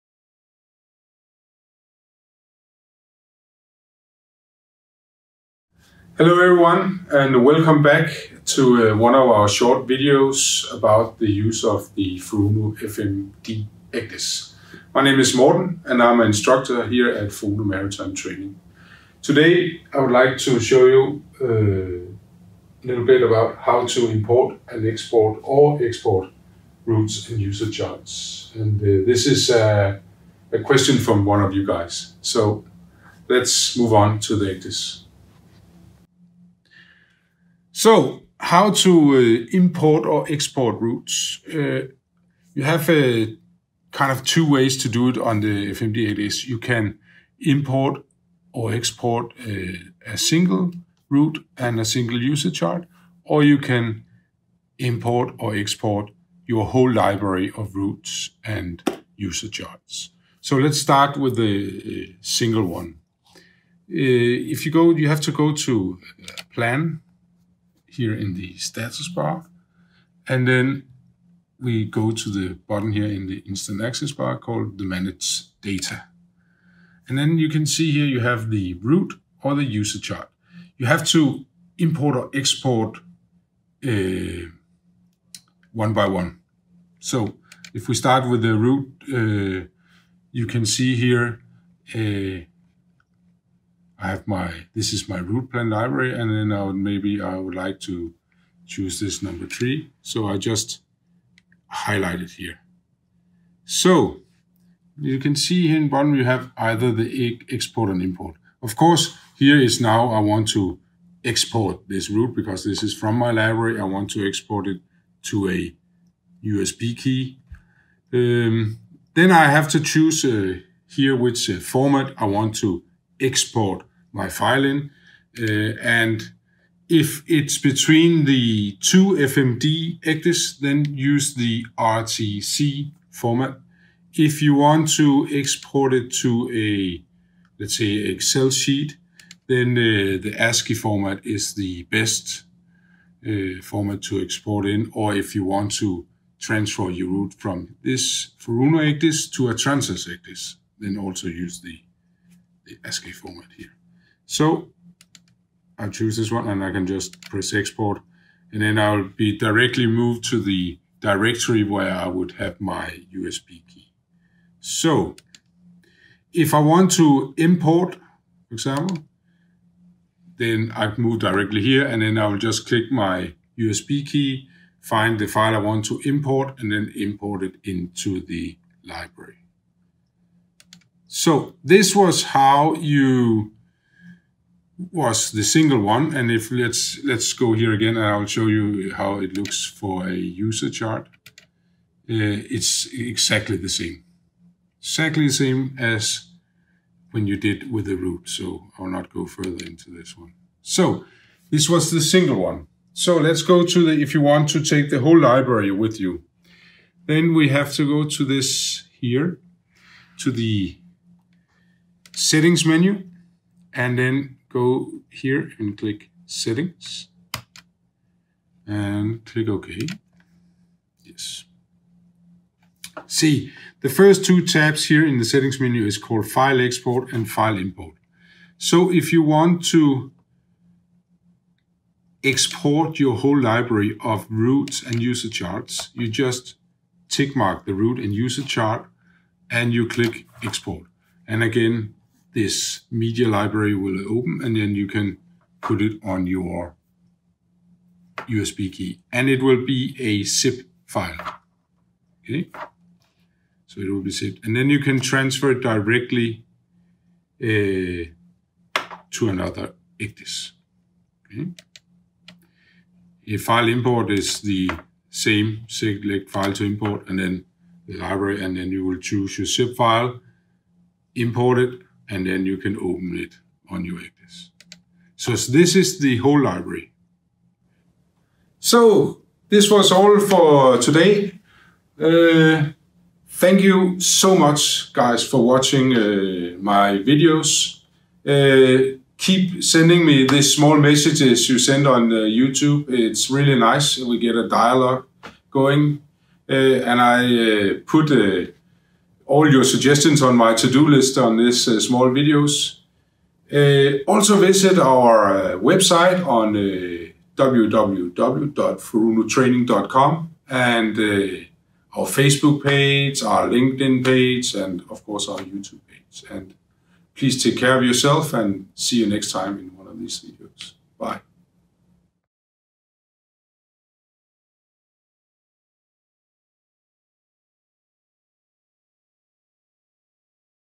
Import or export of routes and user charts | FURUNO ECDIS tutorial
In this video, you will learn how to import or export routes and user charts on the FURUNO FMD ECDIS.

Maritime training for professionals!